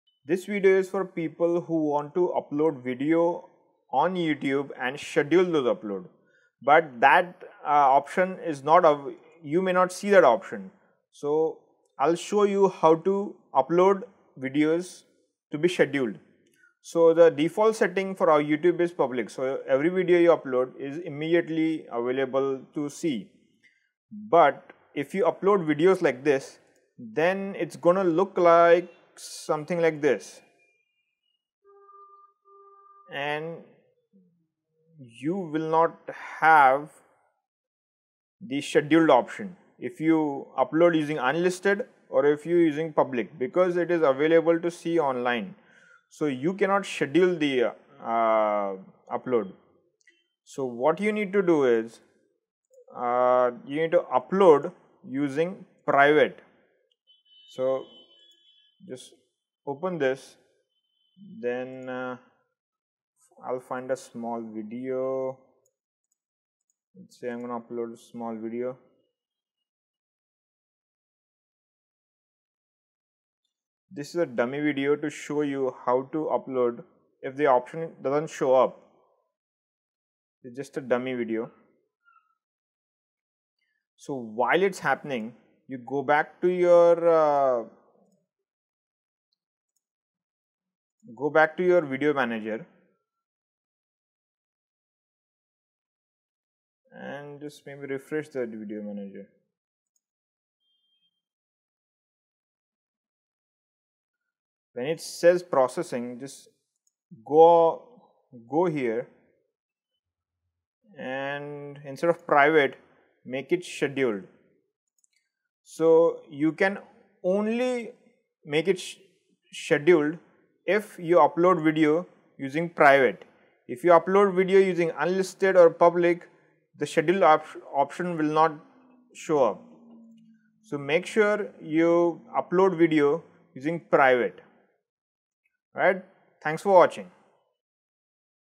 How to fix schedule upload not working on youtube
if your schedule upload is not working on your youtube account then this video will show you how to fix the problem